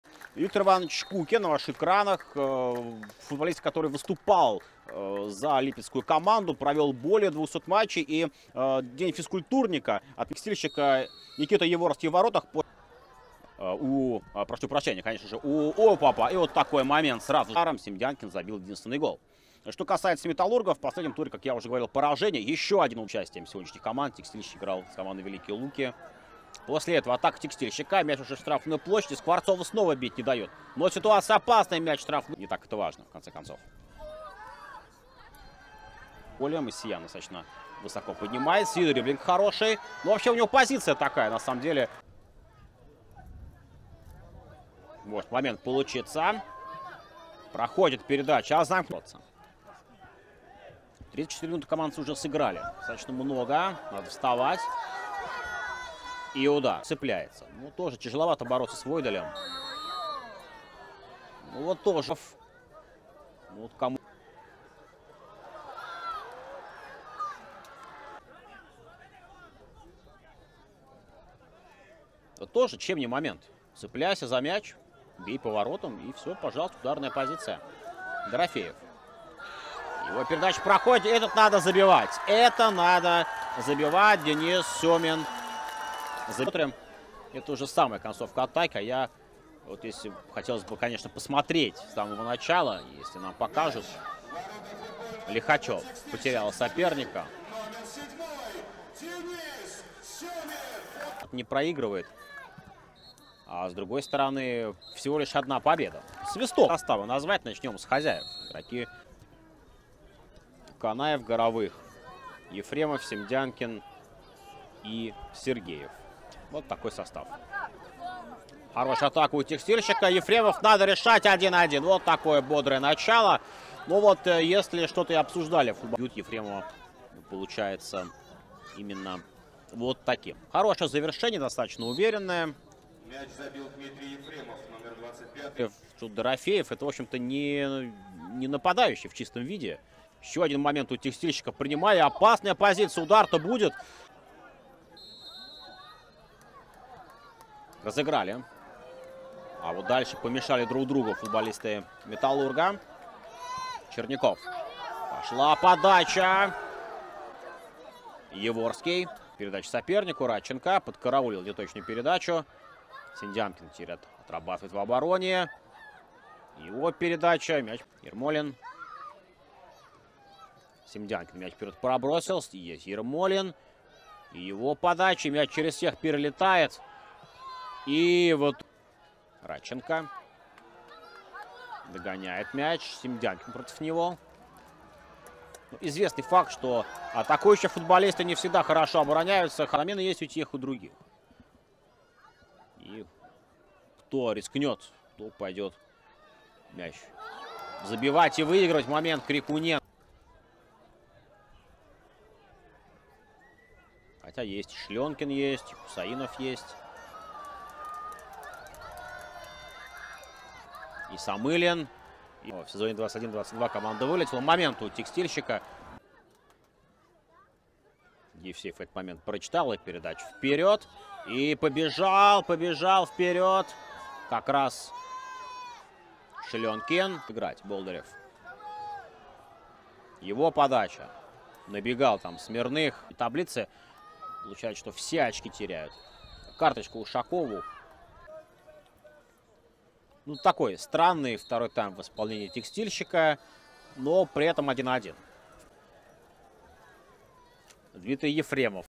Металлург - Текстильщик - 1:1. Обзор
Металлург (Липецк) - Текстильщик (Иваново) - 1:1 (1:0)
Голы: Д.Сёмин, 44 (1:0). Д.Ефремов, 46 (1:1).
Металлург: 23. Матюша. 2. Елеев. 5. Крикуненко. 20. Войдель (к). 25. Макс.Радченко (14. Смирных, 72). 4. Черняков. 8. Д.Пахомов. 6. Н.Дорофеев (15. Е.Ушаков, 61). 13. М.Болдырев. 17. Мосиян (27. Коротких, 84). 7. Д.Сёмин (24. Янушковский, 61).
Текстильщик: 16. Яворский. 4. А.Елисеев (к). 96. Лихачёв. 80. И.К.Климов. 69. Ермолин. 27. Горовых. 17. Канаев (11. Лямзин, 83). 77. Сентюрин (14. В.Сергеев, 46). 25. Д.Ефремов (94. А.Евсеев, 68). 21. Симдянкин (66. Самылин, 83). 92. Ал-й Скворцов (37. Шлёнкин, 88).
Предупреждения: Крикуненко, 18. Н.Дорофеев, 49. Войдель, 54. Лихачёв, 64. В.Сергеев, 81. Лямзин, 87. Е.Ушаков, 90+.
Судьи Ю.Матвеев, Д.Михалёв (оба - Москва), И.Агальцов (Московская область). Инспектор И.Лапидус (Волгоград).
12 августа. Липецк. Стадион Металлург. 1650 зрителей.